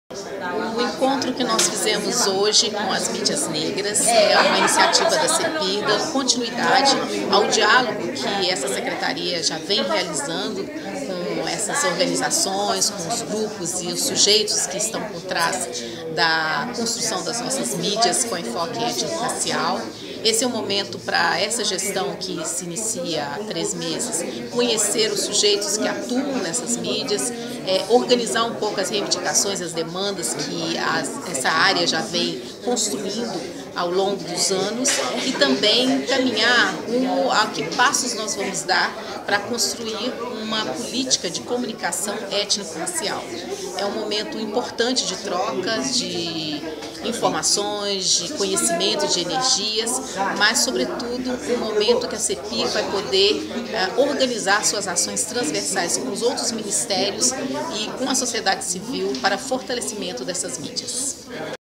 Ministra Nilma Lino Gomes em encontro com comunicadores e mídias negras
Hoje (08/04), o governo federal, através da SEPPIR, promoveu uma mesa de diálogo com comunicadores e mídias negras. O principal tema debatido foi o fortalecimento de políticas públicas que garantam uma MídiaSemRacismo. A ministra Nilma Lino Gomes (Secretaria de Políticas de Promoção da Igualdade Racial) fez parte da mesa e destacou o papel social das mídias étnicas neste processo.